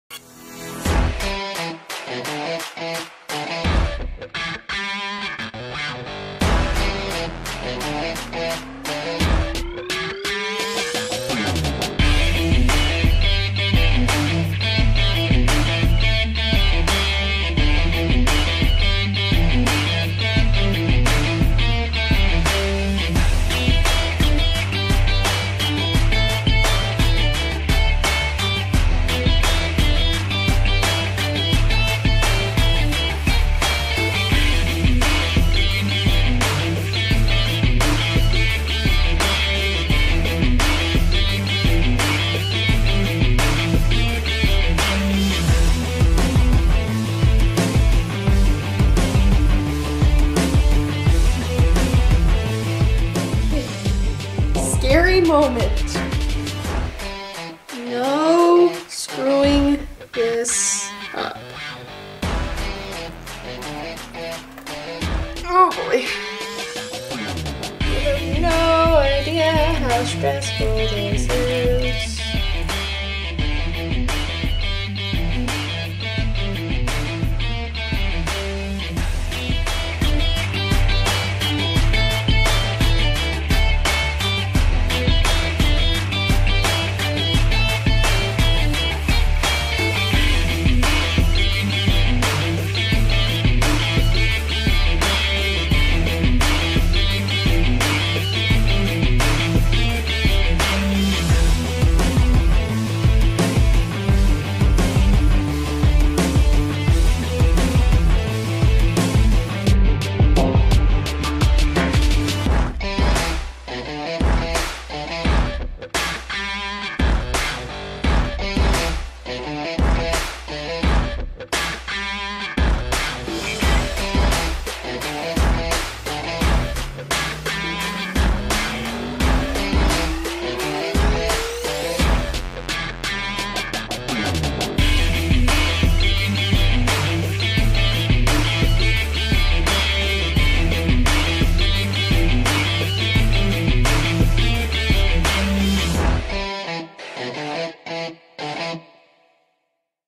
Roxanne Wolf FNAF Cosplay Commission PART 2
Roxanne Wolf from, Five Nights at Freddy's, is coming to life in our studio.
This is the 2nd video in this commission series and has been made to show our commissioner the work that has been done so far.
The skirt and halter top have been paired with their black designs and the sewing has commenced.